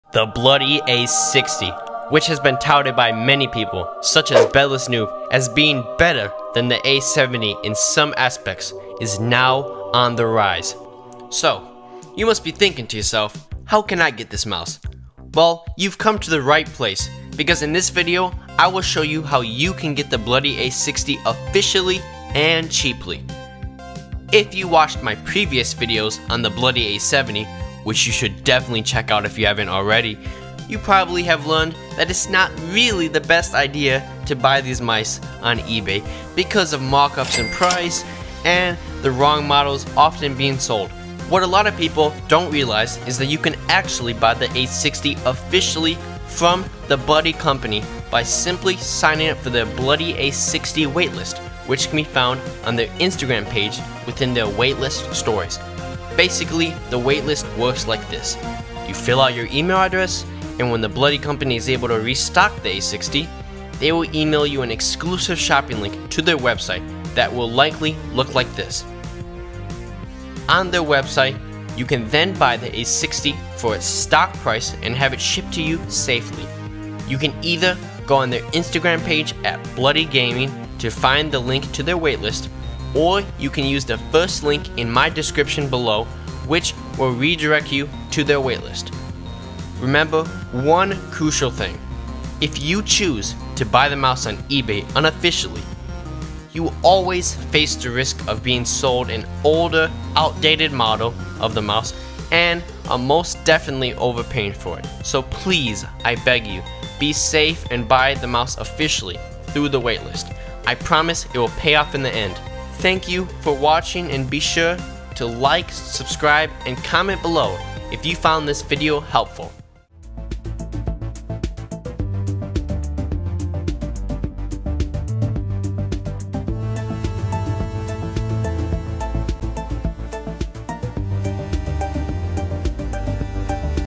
How to Buy the Bloody A60
In this video, I explain how anyone can officially buy the A4tech Bloody A60 Lightstrike Gaming mouse. The Bloody a60 is an exceptional dragclicking mouse that can get very high cps in Minecraft; this allows for people to godbridge and moonwalk bridge in Minecraft Hipixel Bedwars and MLG Rush on other servers. Youtubers, such as @Bedless Noob, have reviewed this mouse and noted that it has some advantages in certain areas over the more famous Bloody A70 Mouse. The best way to buy and get the Bloody A60 is by purchasing it through Bloody gaming directly VIA a waitlist. Most people that godbridge in Minecraft use the bloody A60 or the bloody A70 because they both can dragclick unlike most other mice. The light strike switches combined with the design of the A60 allows for it to register virtually all clicks and even double click consistently, which can help with butterfly clicking. Also let me know if you can godbridge in bedwars and what is your favorite drag clicking mouse.

Mouse: A4Tech Bloody A60
MIC: Rode NT1
Edited On: FCPX
Recorded With: Logic Pro X